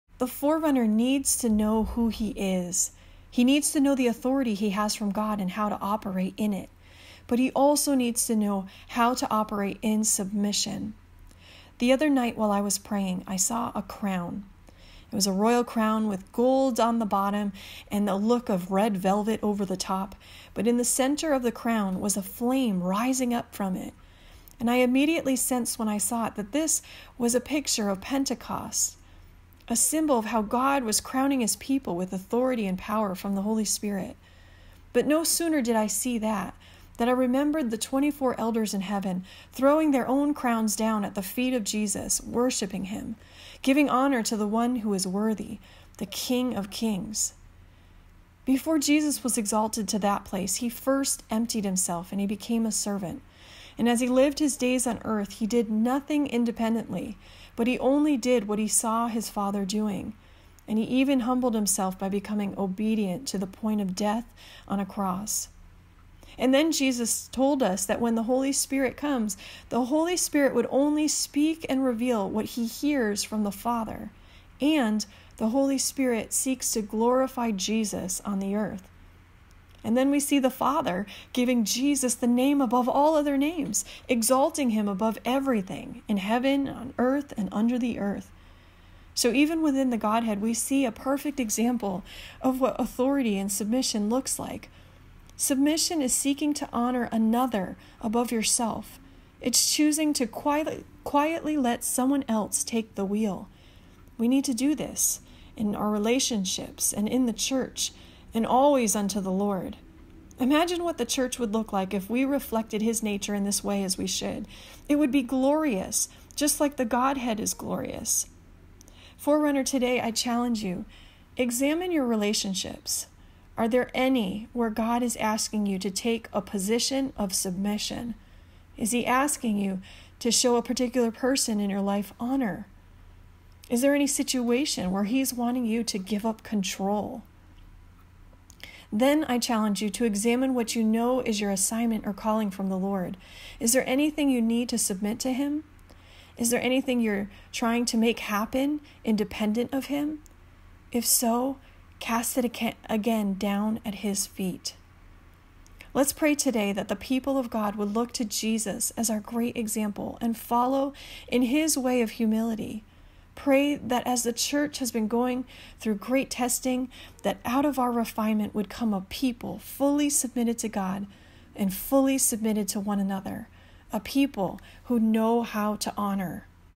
Day 21 of 40 Prayer Initiative
The forerunner needs to know how to operate in submission even as he needs to operate in authority.

We see within the godhead a perfect example of what authority and submission looks like.
Phil. 2:7-10, John 5:19, John 16:13-15

Submission is seeking to honor another above yourself. It’s choosing to quietly let someone else “take the wheel”.  Imagine what the church would look like if we reflected his nature in this way as we should.  It would be glorious!

Challenge:
-Examine your relationships. Are there any in which God is asking you to take a position of submission?  Is he asking you to show honor to a particular person in your life?  Any situation where he is wanting you to give up control?
-Examine what you know is your assignment and calling from the Lord. Is there anything you are trying to make happen independent of him?  If so, cast it again at his feet.

Pray:
That the people of God would look to Jesus as our great example and follow in his way of humility.
That the church, in this time of refinement, would come out a people fully submitted to God, fully submitted to one another.  A people who know how to honor.